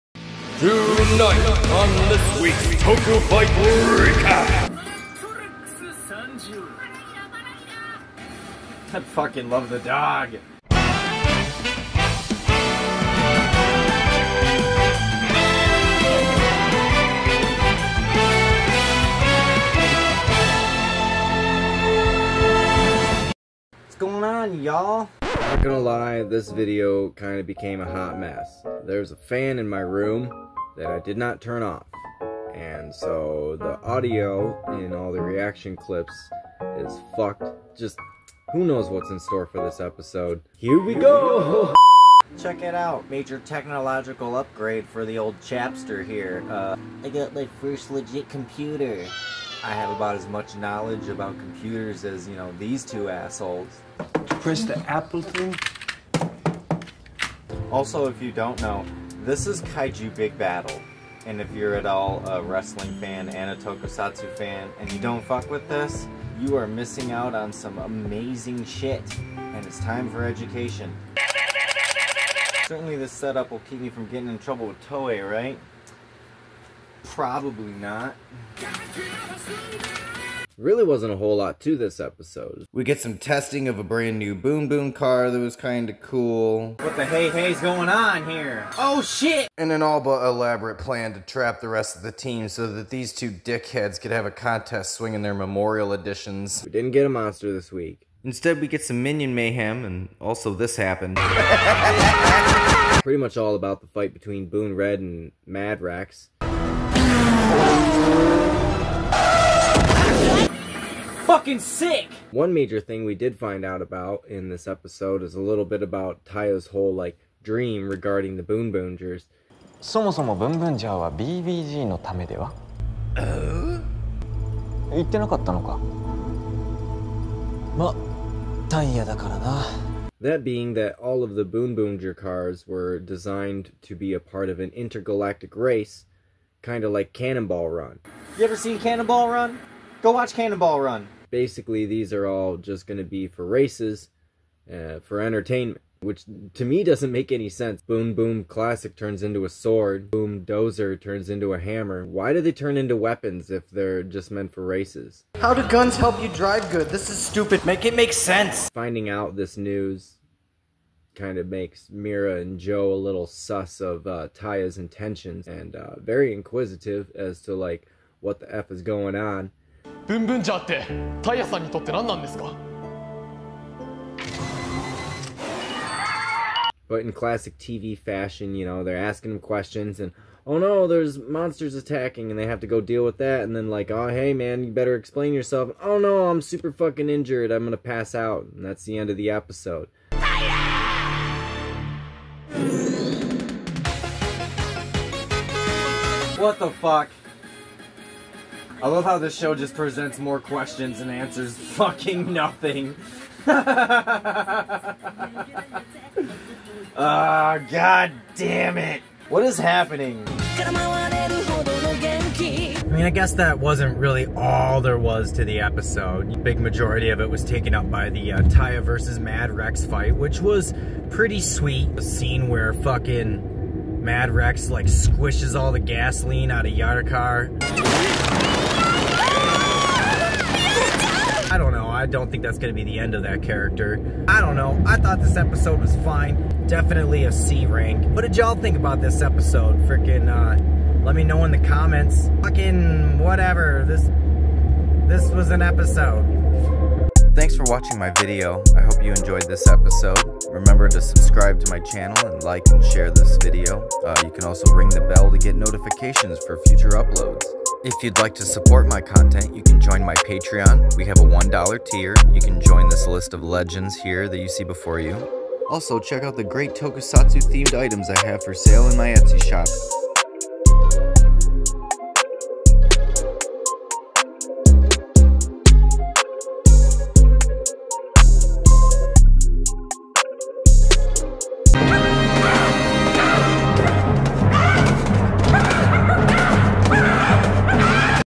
DID IT TRAP? Boonboomger Ep.8 REVIEW
This video is a mess of audio issues, and editing but it's only 5 minutes idk this episode wasn't much really and I had the pleasure of being sick all week. Hope you enjoy this short one as we dive into more Mad Rex vs. Bun Red in this week's TOKU FIGHT RECAP!